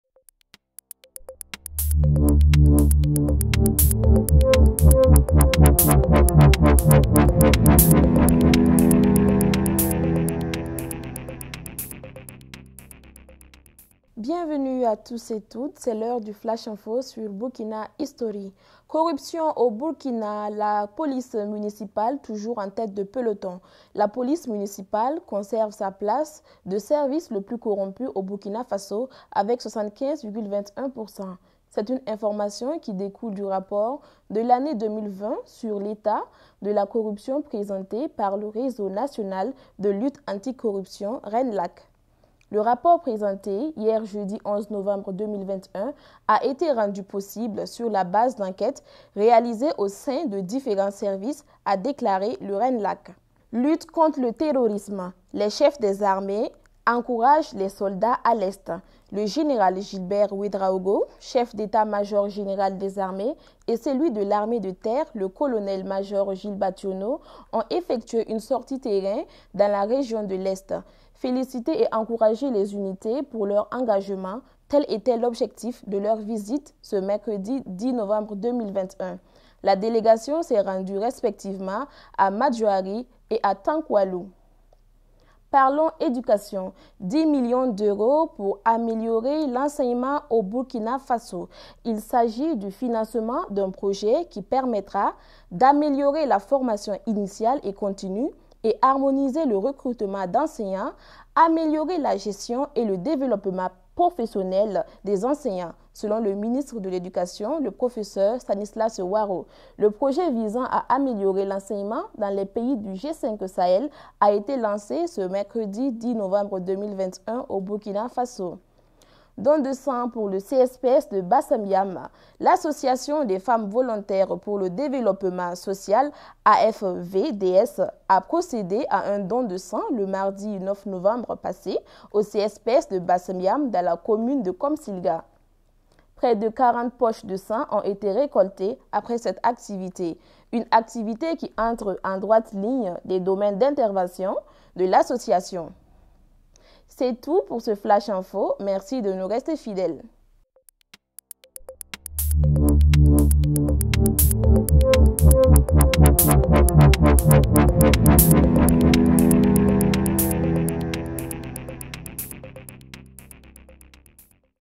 FLASH / Corruption / Lutte contre le terrorisme / Éducation / Don de sang pour le CSPS de Basm Yam /
Corruption au Burkina : La police municipale toujours en tète de peloton !
La police municipale conserve sa place de service le plus corompu au BF avec 75,21%.
C’est une information qui découle du rapport, de l’année 2020, sur l’état de la corruption présenté par le réseau national de lutte anti corruption (RENLAC).
Le rapport présenté hier jeudi 11 novembre 2021 a été rendu possible sur la base d’enquêtes réalisées au sein de différents services, a déclaré le RENLAC.

Lutte contre le terrorisme: Les Chefs des armées encouragent les soldats à l’Est
Le Général Gilbert Ouedraogo, Chef d’état-major général des Armées et celui de l’armée de terre , le Colonel-major Gilles Bationo ont effectué une  sortie terrain dans la région de l’Est.
« Féliciter et encourager les unités pour leur engagement », tel etait  l’objectif de leur visite ce mercredi 10 novembre 2021  .
la délégation s’est rendue respectivement à Madjoari et à Tankoualou.

Éducation : 10 millions d’euros pour améliorer l’enseignement au Burkina
Il s’agit du financement d’un projet qui permettra « d’améliorer la formation initiale et continue et harmoniser le recrutement d’enseignants, améliorer la gestion et développement professionnel des enseignants» selon le ministre de l'éducation, le Pr  Stanislas  Ouaro.
Le projet visant à « améliorer l’enseignement dans les pays du G5 Sahel» a été lancé ce mercredi 10 Novembre 2021 au Burkina Faso.

Don de sang pour le CSPS de Basm-Yam
L’association des femmes volontaires pour le développement social (AFVDS) a procédé à un don de sang le mardi 09 Novembre passé au CSPS de Basm yam, dans la commune de Komsilga.
Près de 40 poches de sang ont été récoltés après cette activité .
Une activité qui entre en droite ligne des domaines d’intervention de l'association